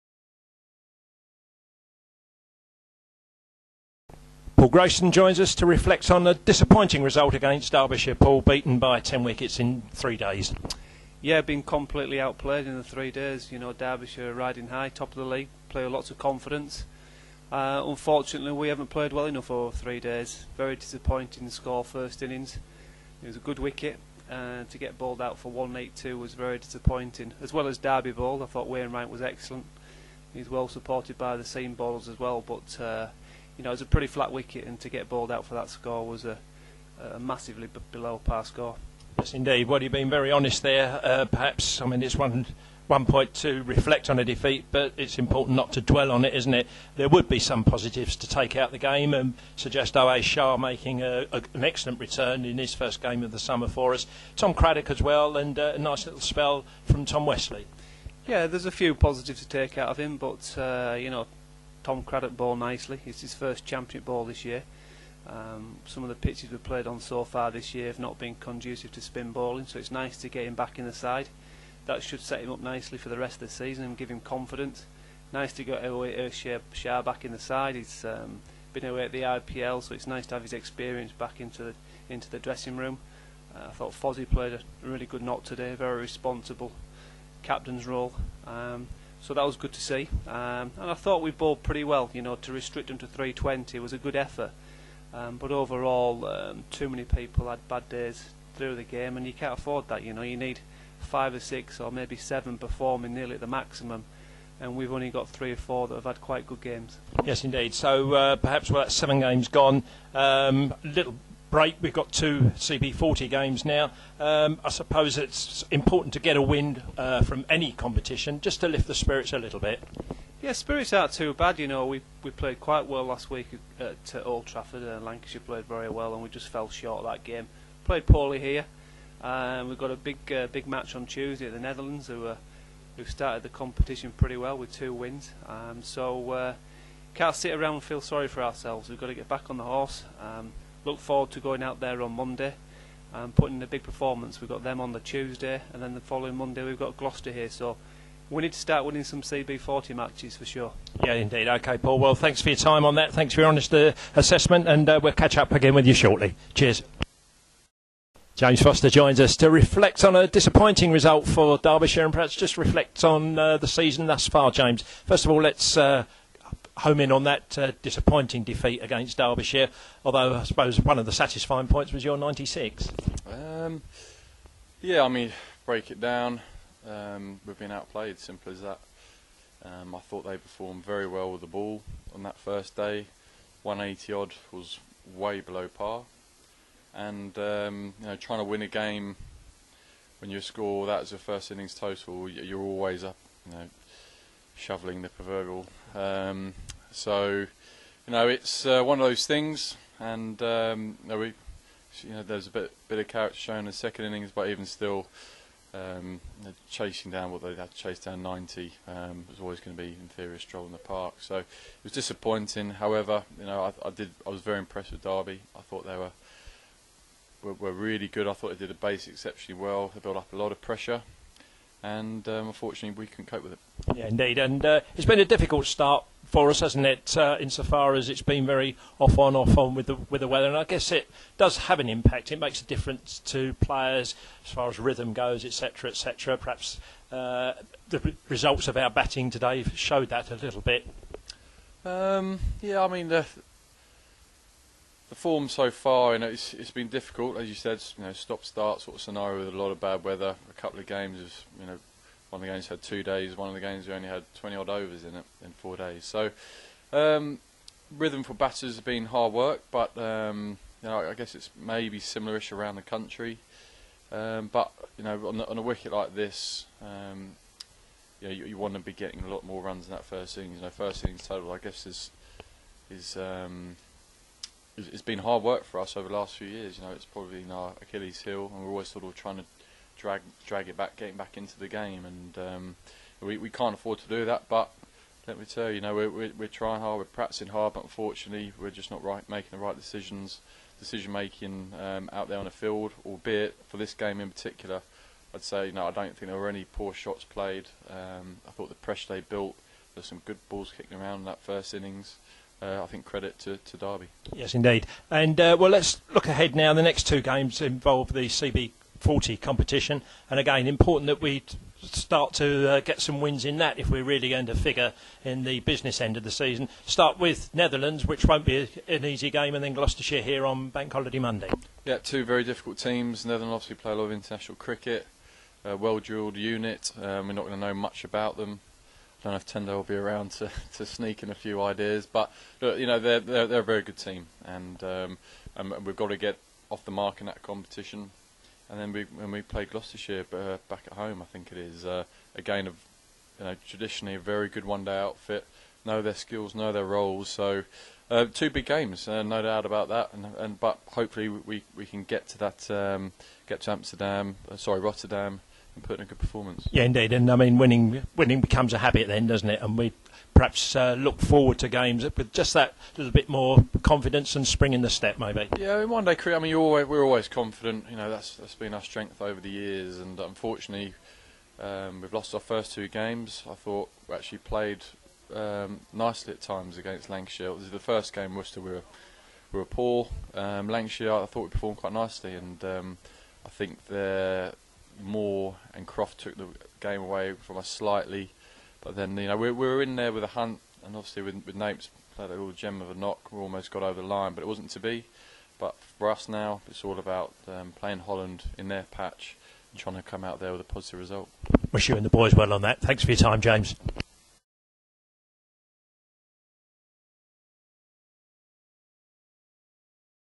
Paul Grayson & James Foster React to Derbyshire defeat
Essex's Paul Grayson & James Foster react after the LV= County Championship defeat to Derbyshire at The Ford County Ground.